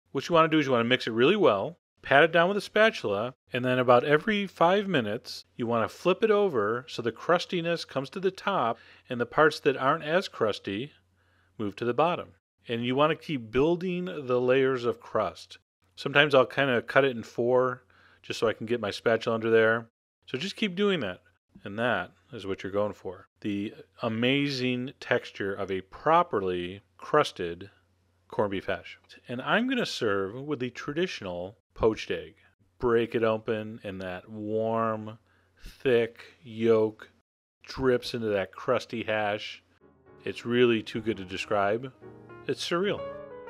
How to Make Chef John's Corned Beef Hash
Learn the secrets to crafting Chef John's legendary Corned Beef Hash! In this step-by-step tutorial, discover how to transform simple ingredients like corned beef, potatoes, and onions into a mouthwatering breakfast or brunch favorite. Chef John's expert tips and techniques ensure your hash turns out perfectly every time. Whether you're a seasoned cook or a beginner, this recipe is sure to become a staple in your recipe rotation.